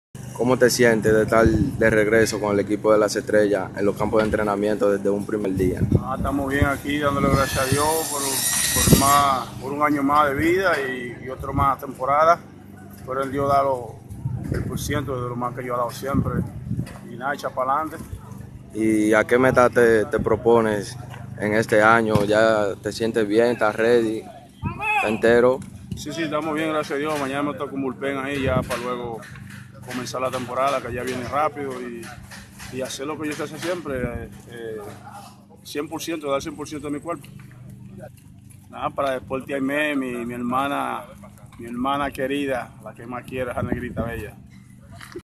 @deportesxaime
Entrevista a Edward Valdez lanzador @eobaseballclub